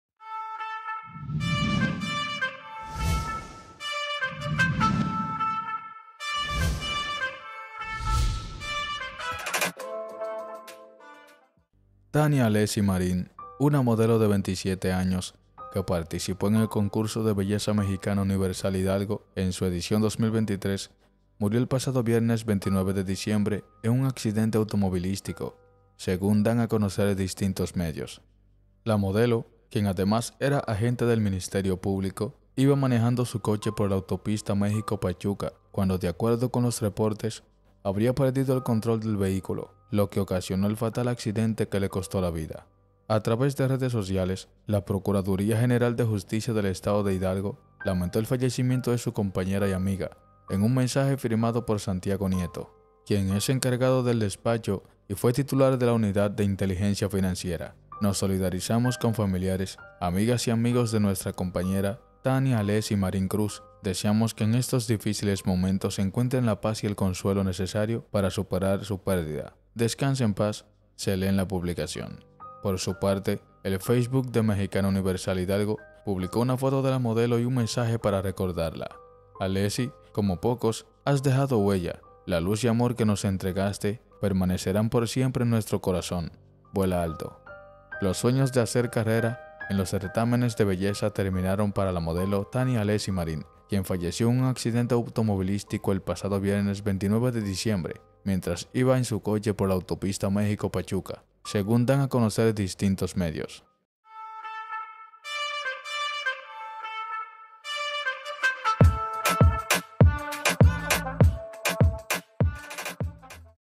MUERE concursante de Mexicana Universal a los 27 años en ACCIDENTE
Tania Alessi Marín, quien formaba parte de Mexicana Universal Hidalgo, falleció en un accidente automovilístico el pasado viernes.